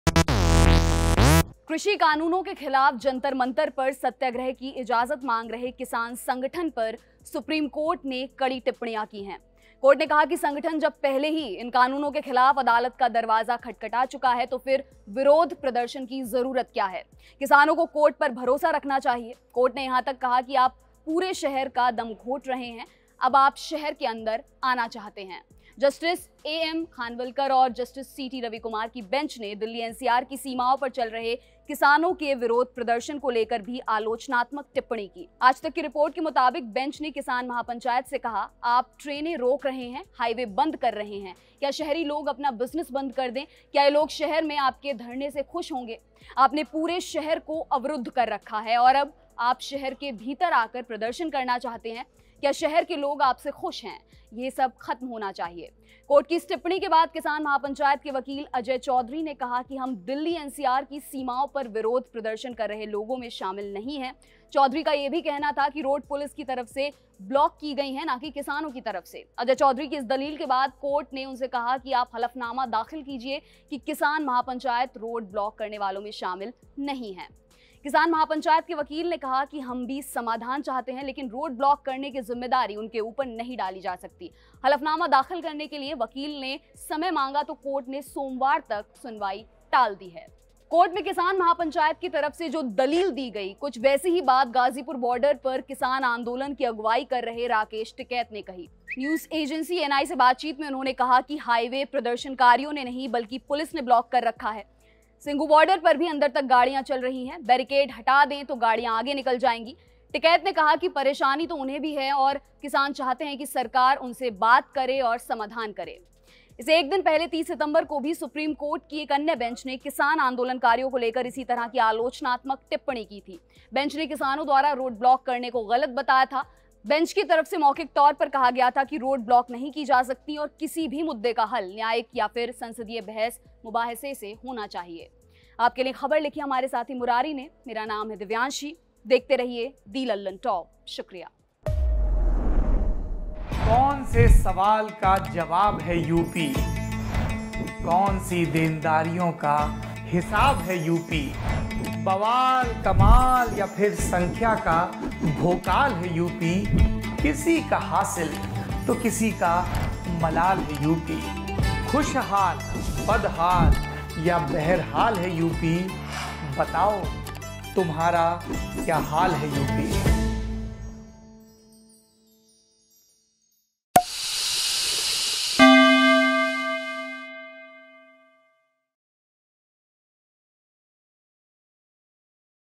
सुप्रीम कोर्ट ने किसान प्रर्दशनकारियों को फटकारा, कहा - आप पूरे शहर का दम घोंट रहे हैं!
The Supreme Court has made critical  comments on the farmers' organization seeking permission for 'Satyagraha' at Jantar Mantar against agricultural laws. The court said that when the organization has already approached the court against these laws, then what is the need of protesting. Farmers should have faith in the court. Watch the video for more details.